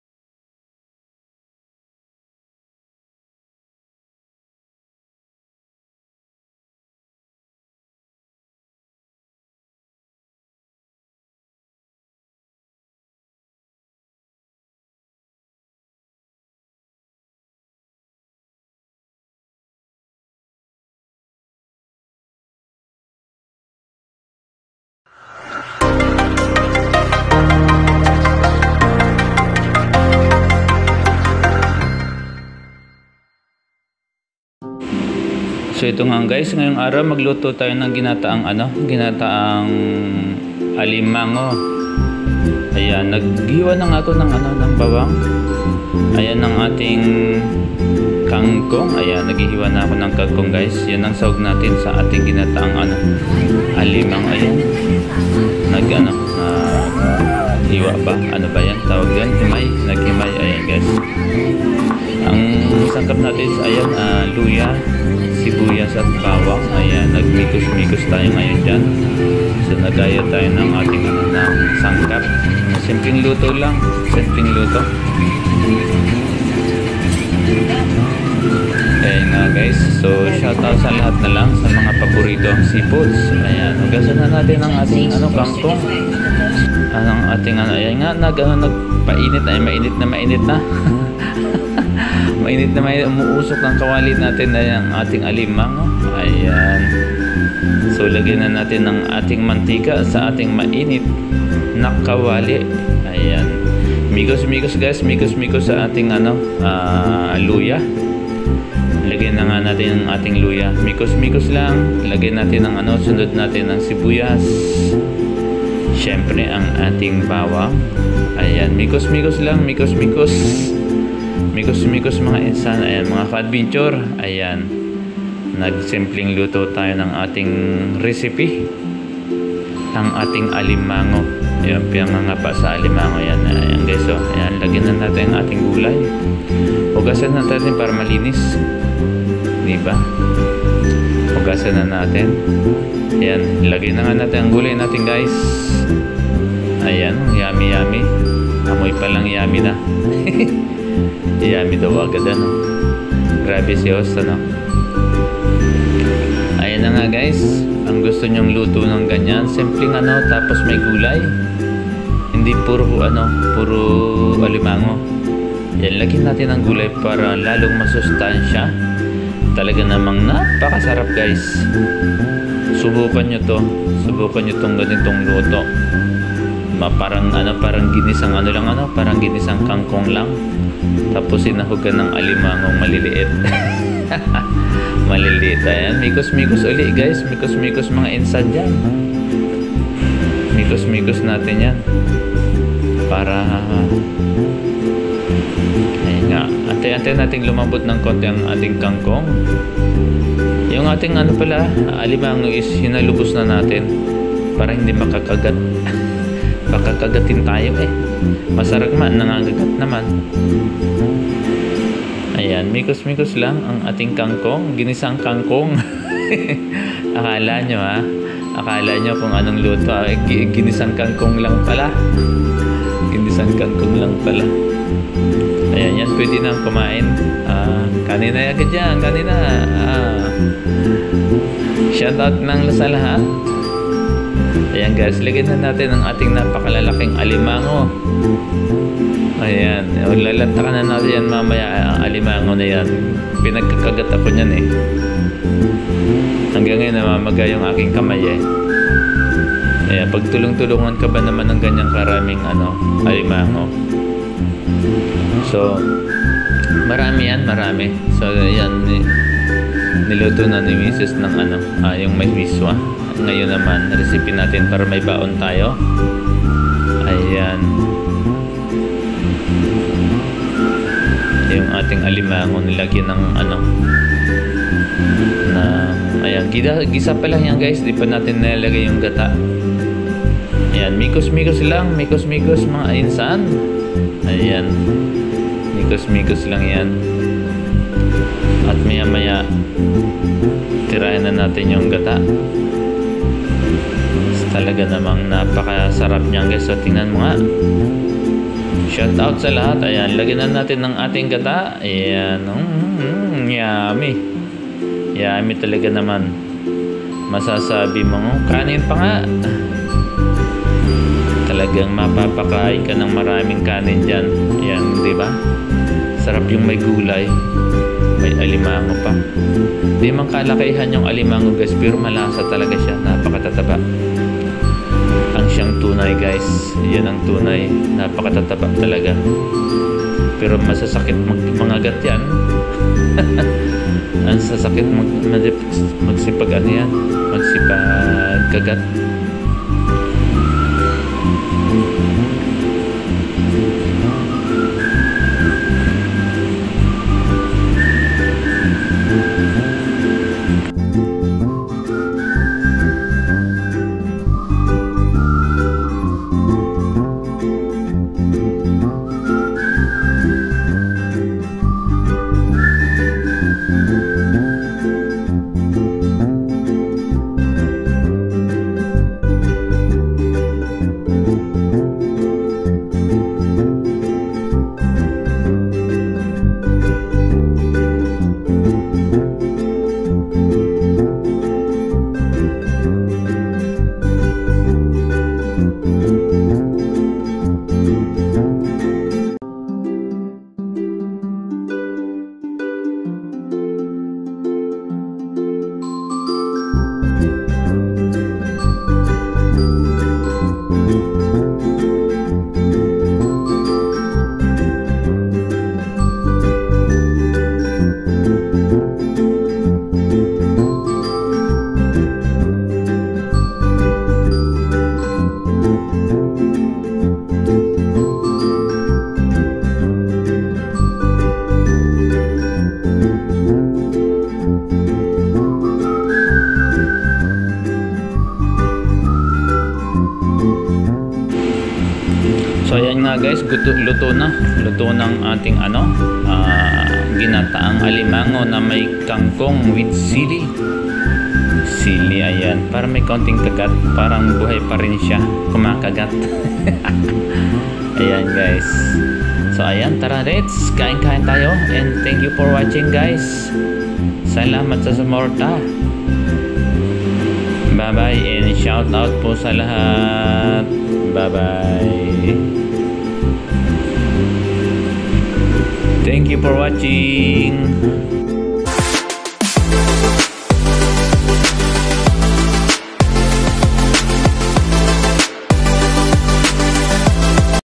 GINATAANg alimango/kangkong# YummY foods
paano pasarapin ang luto nyo nang alimango with sempleng luto sa gata... panoòrin